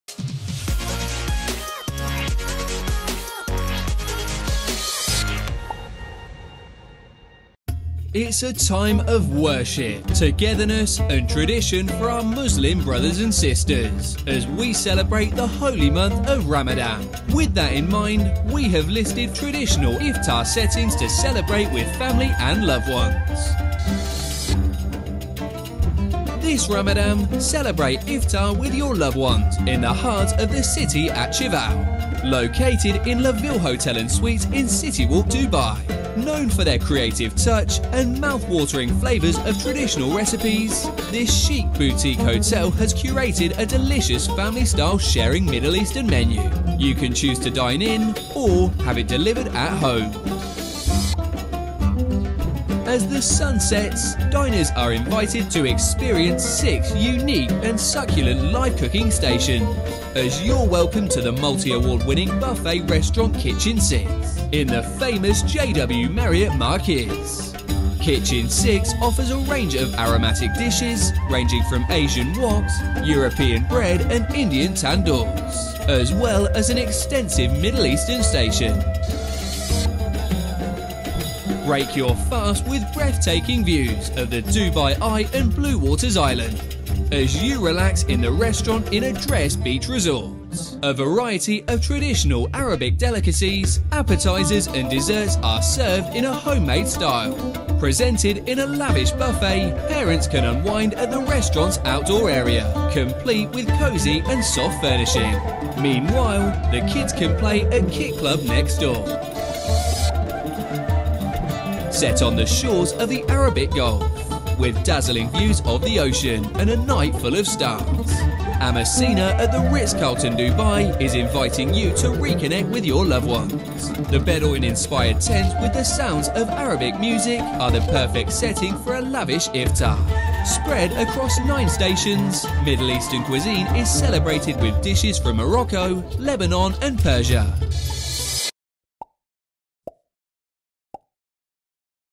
Iftar in Dubai | Ramadan 2021
As we celebrate the holy month of Ramadan, it is a time of prayer, togetherness, and tradition for our Muslim brothers and sisters.

Celebrate Iftar with your loved ones in the heart of the city this Ramadan at Chival.
Located in City Walk Dubai's La Ville Hotel & Suites.
This trendy boutique hotel has curated a delicious family-style sharing middle-eastern menu, known for their innovative touch and mouthwatering flavors to conventional recipes.
You can eat in or have it delivered to your house.

At the multi-award-winning buffet restaurant Kitchen6 in the famed JW Marriott Marquis, diners are invited to experience six succulent live-cooking stations as the sunsets.
Kitchen6 serves a variety of fragrant dishes, including Asian woks, European bread, and Indian tandoors, as well as a large Middle Eastern station.

At The Restaurant in Address Beach Resort, you can break your fast with spectacular views of the Dubai Eye and the Blue Waters Islands. A luxurious buffet with live carving stations serves a selection of traditional Arabic delicacies, appetizers, and desserts in a homemade theme. While the kids play Qix club right next door, parents can relax in the restaurant's outdoor area, which is comfortable and softly furnished.

Set on the Arabian Gulf's shores, with breathtaking views of the sea and a night filled with stars.
The Ritz-Carlton, Dubai's Amaseena invites you to reconnect with loved ones. The Bedouin-inspired tents, complete with Arabic music, are the ideal setting for an extravagant Iftar.
Middle Eastern cuisine is celebrated through nine stations, with dishes from Morocco, Lebanon, and Persia.